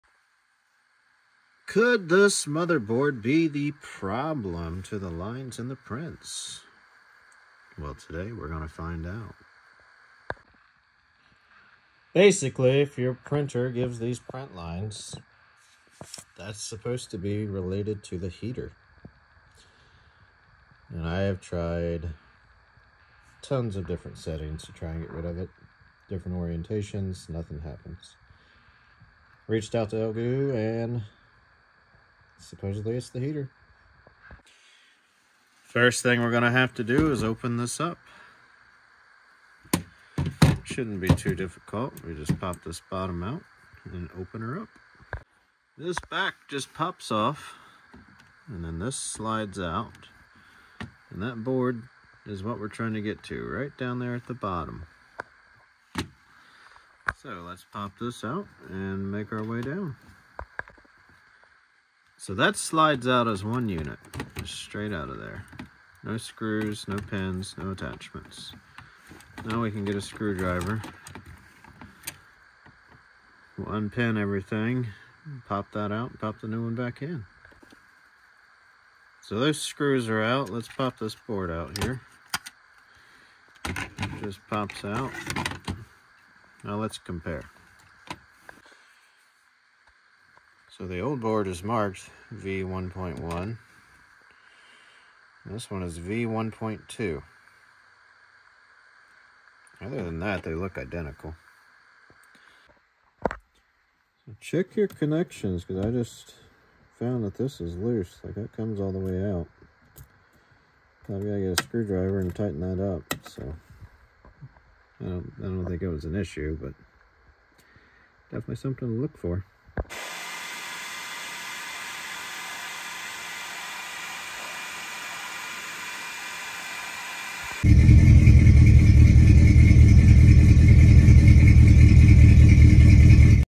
Elegoo heater motherboard replacement
Replacing the motherboard on the elegoo heater in my Saturn 4 ultra. The heater can cause unwanted print lines in the final print resulting in a failure.